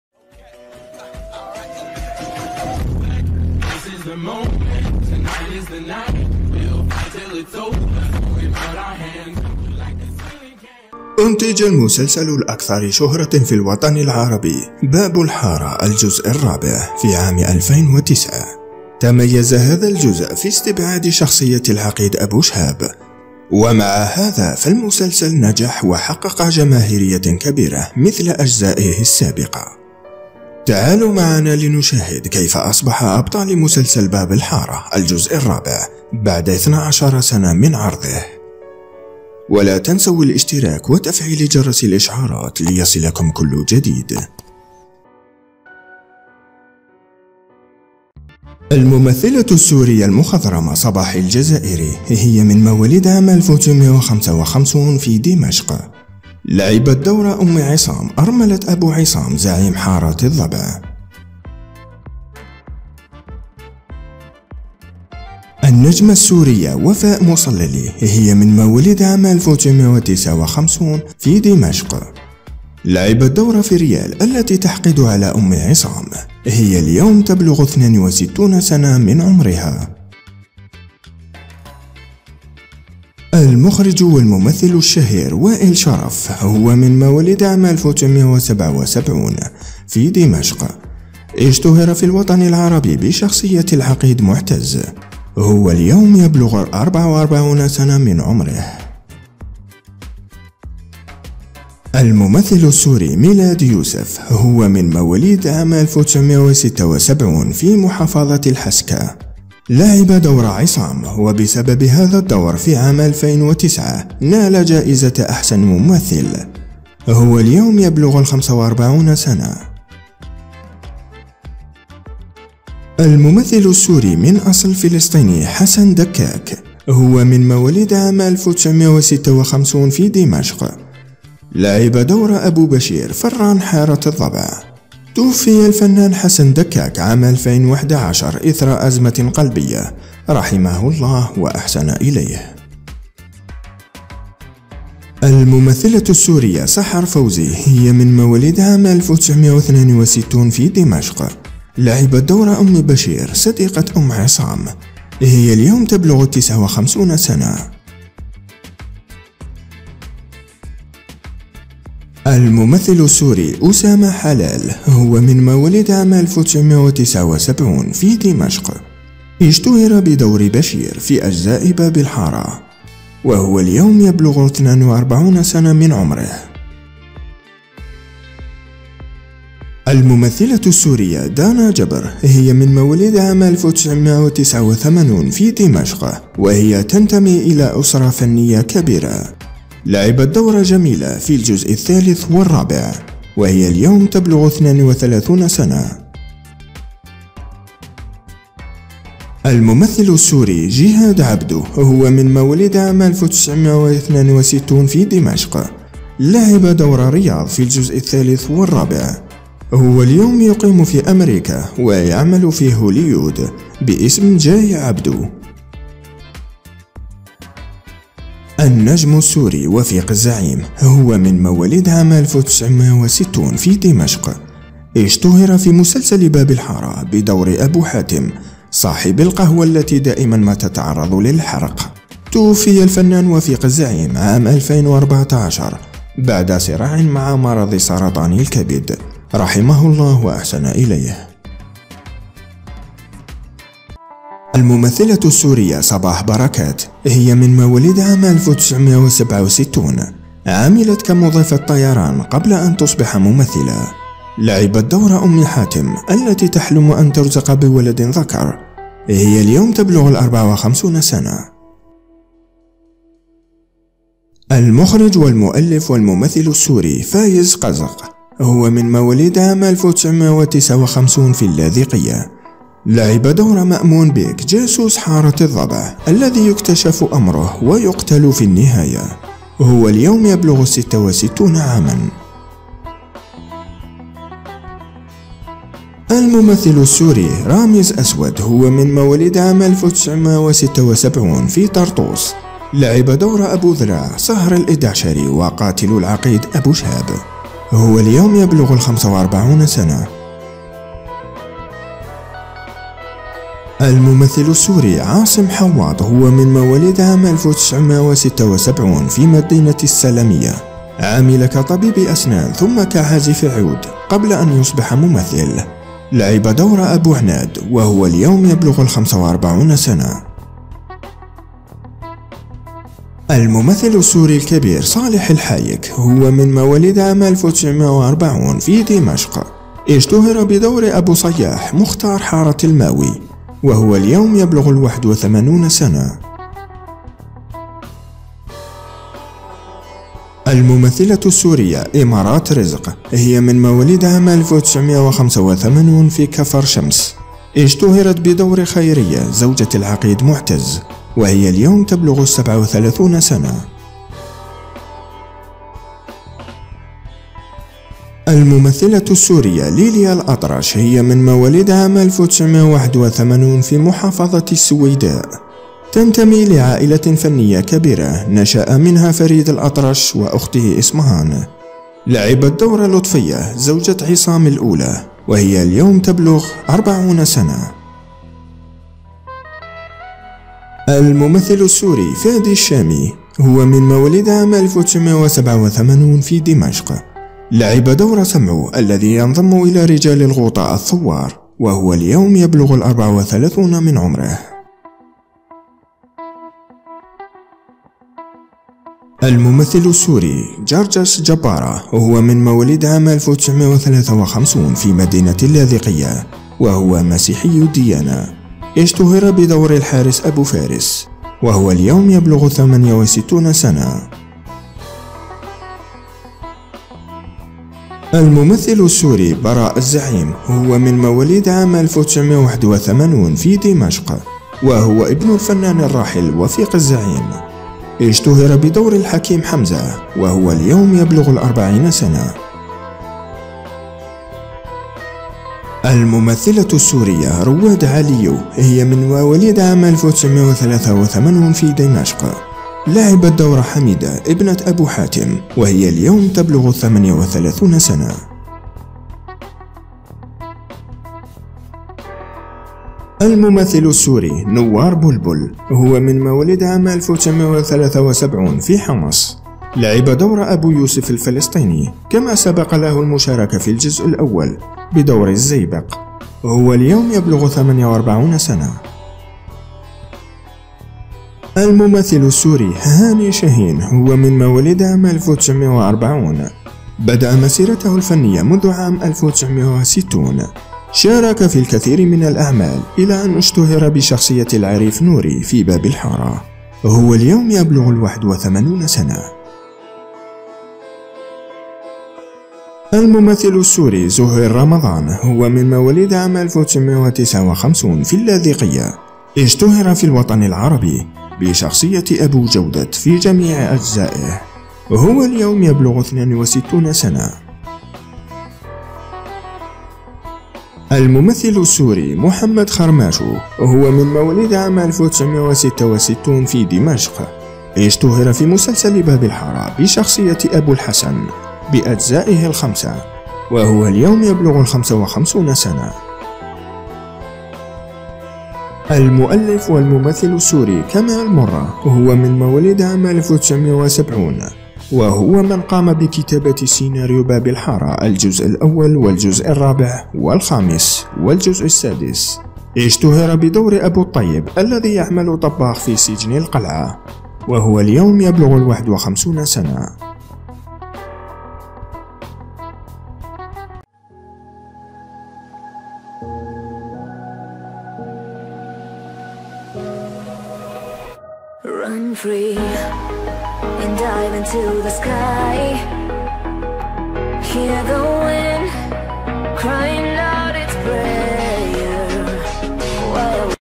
لن تصدق كيف أصبح أبطال مسلسل | باب الحارة 4 | بعد 12 سنة !!
أنتج المسلسل الأكثر شهرة في الوطن العربي باب الحارة الجزء الرابع في عام 2009 , تميز هذا الجزء في استبعاد شخصية العقيد أبو شهاب , ومع هذا فالمسلسل نجح وحقق جماهرية كبيرة مثل أجزائه السابقة ,تعالوا معنا لنشاهد كيف أصبح أبطال مسلسل باب الحارة الجزء 4 بعد 12 سنة من عرضه .

لو أعجبكم الفيديو اعملوا مشاركة وإعجاب للفيديو واشتركوا فى القناة لكي تتابعو كل جديد : اشتراك ❌ اللايك 💓🆂🆄🅱🆂🅲🆁🅸🅱🅴💓